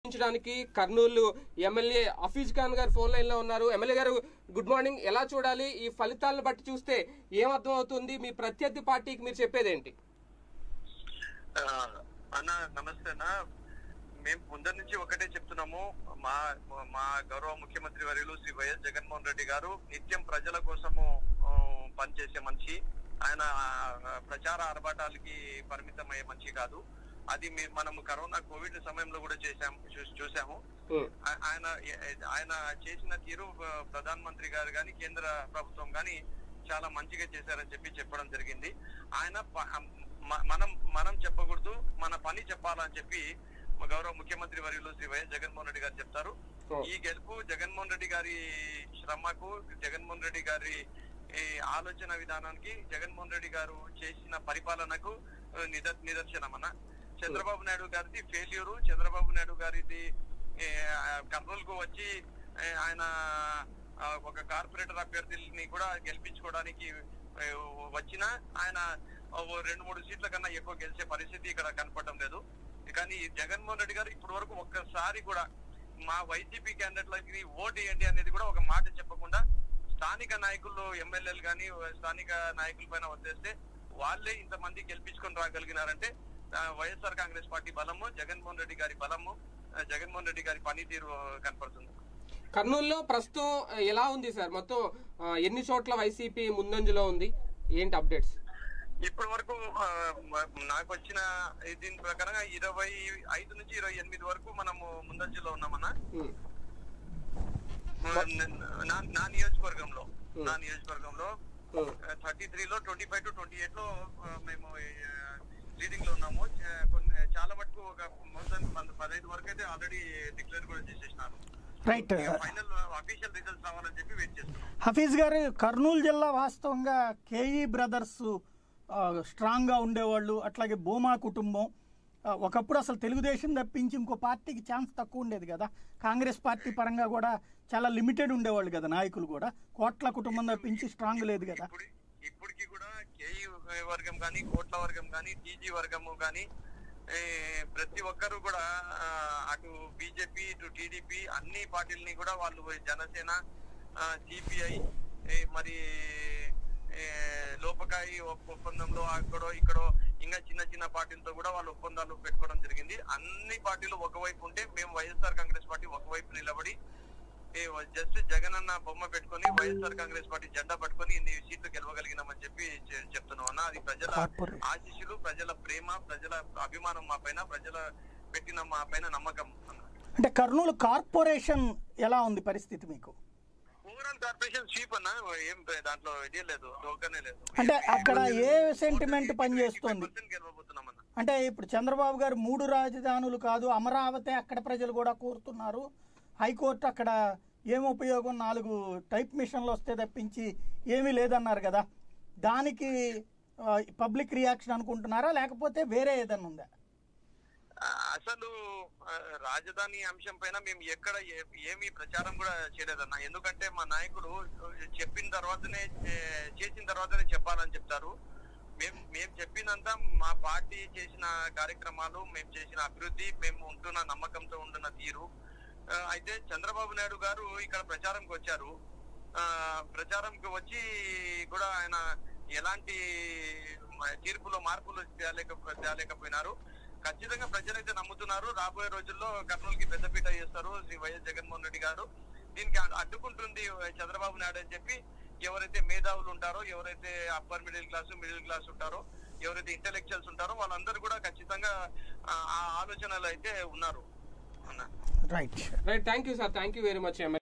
YSRCP MLA Hafeez Khan | Ys Jagan | Municipal Elections | వైసీపీ గెలుపుపై ఎమ్మెల్యే రియాక్షన్ | PDTV
వైసీపీ గెలుపుపై ఎమ్మెల్యే రియాక్షన్